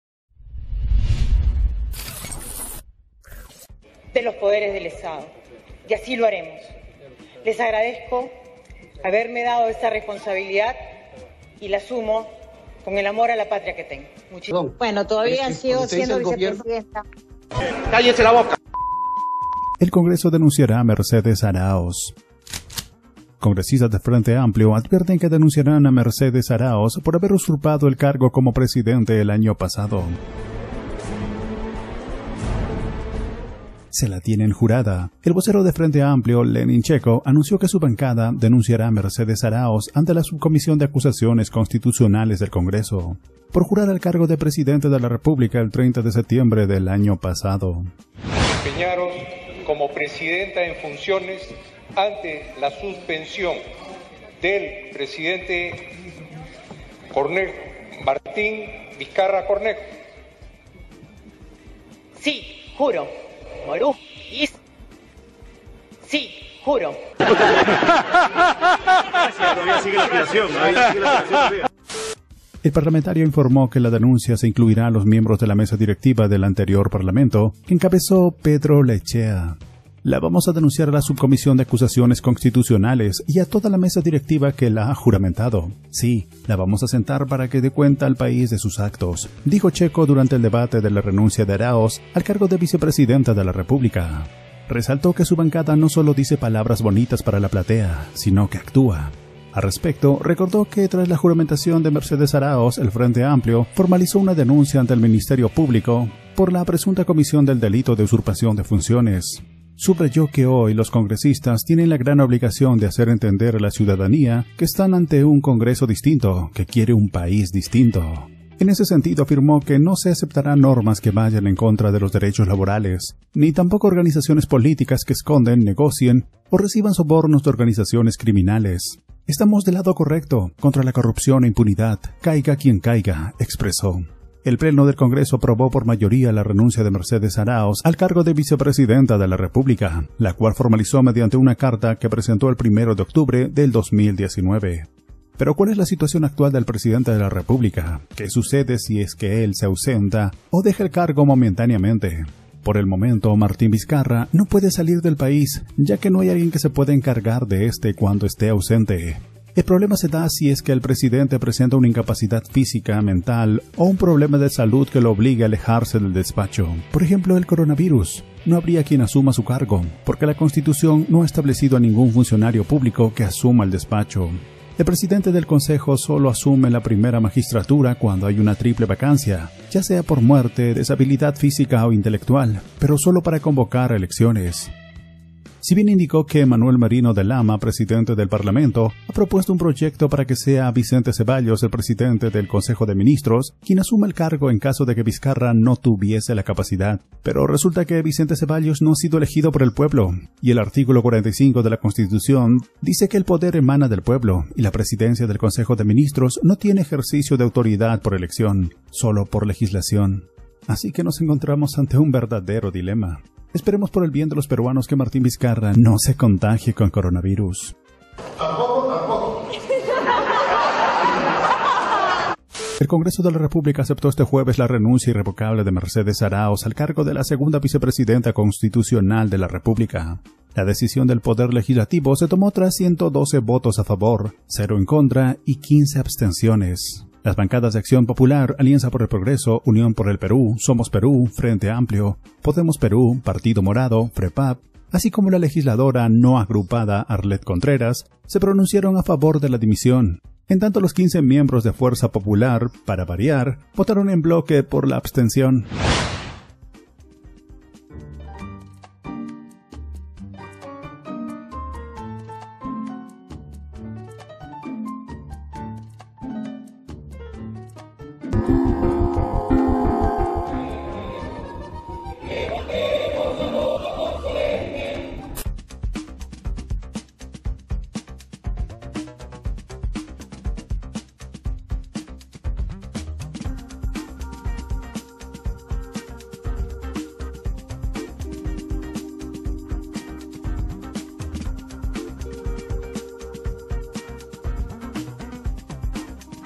CONGRESO ANUNCIA QUE DENUNCIARAN A MERCEDES ARAOZ POR USURPACION DE FUNCIONES TRAS ACEPTAR RENUNCIA
El parlamentario informó que la denuncia se incluirá a los miembros de la Mesa Directiva del anterior Parlamento que encabezó Pedro Olaechea.

“La vamos a denunciar a la Subcomisión de Acusaciones Constitucionales y a toda la Mesa Directiva que la ha juramentado. Sí, la vamos a sentar para que dé cuenta al país de sus actos”, dijo Checco durante el debate de la renuncia de Aráoz al cargo de vicepresidenta de la República.